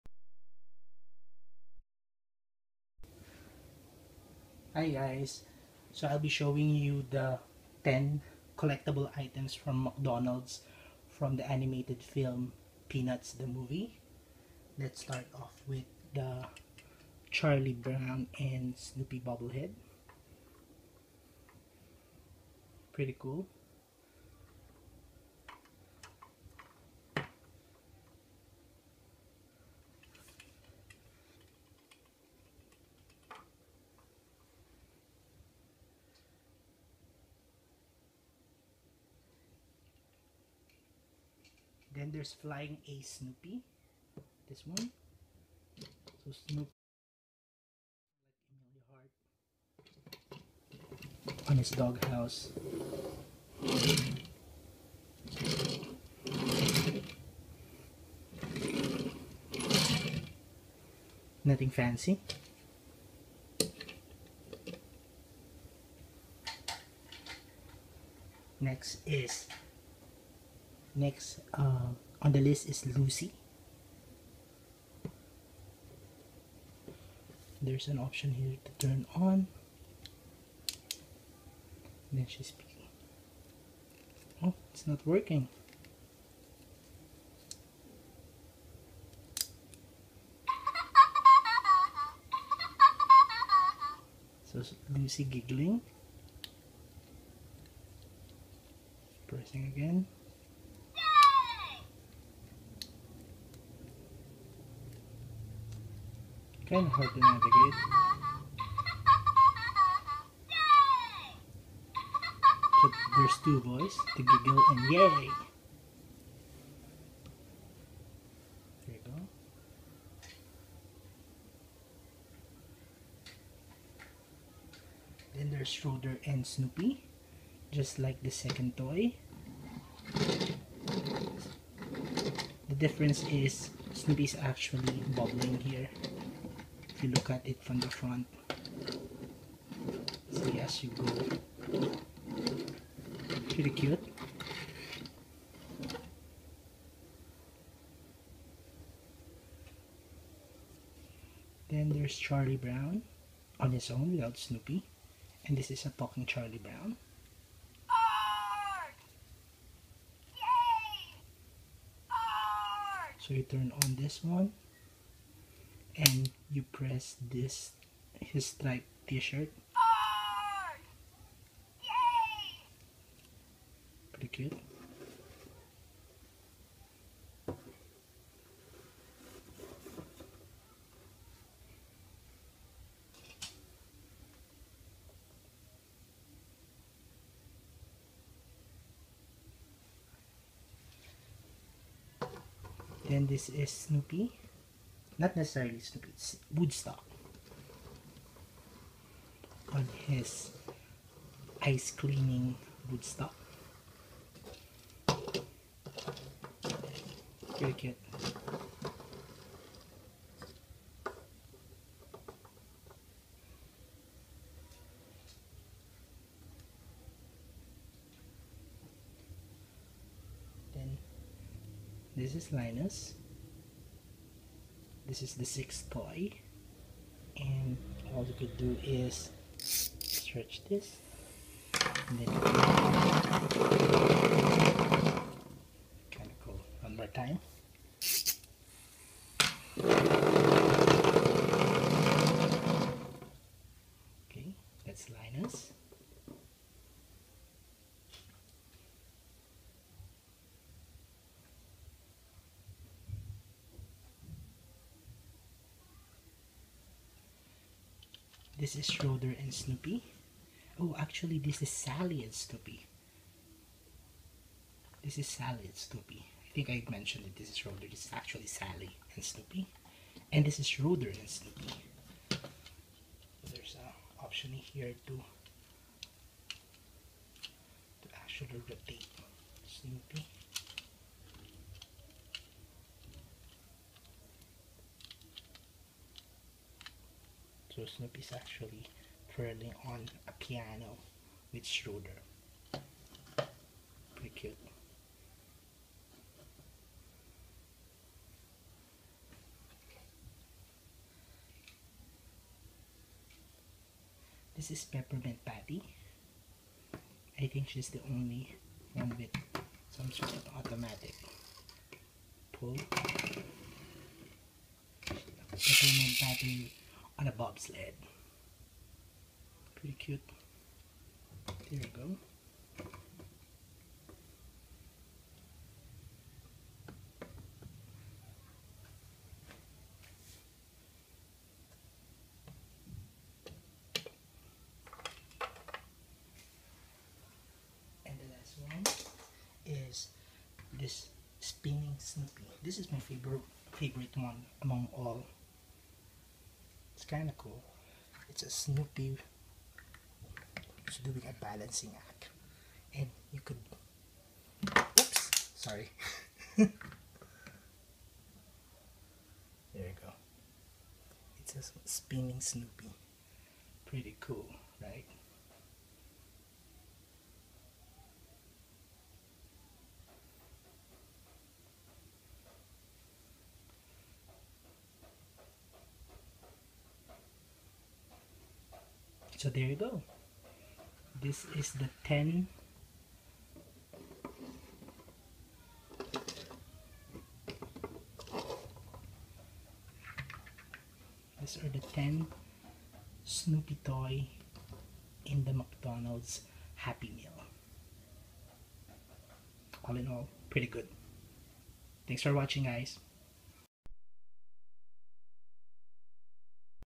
McDonald's The Peanuts Movie Happy Meal Toys
"The Peanuts Movie" Toys from McDonald's Happy Meal. Collect all 10! (Charlie Brown, Lucy, Flying Ace Snoopy, Linus, Sally and Snoopy, Ice Cleaning Woodstock, Peppermint Patty, Schroeder and Snoopy, Charlie Brown and Snoopy Bobble and Spinning Snoopy).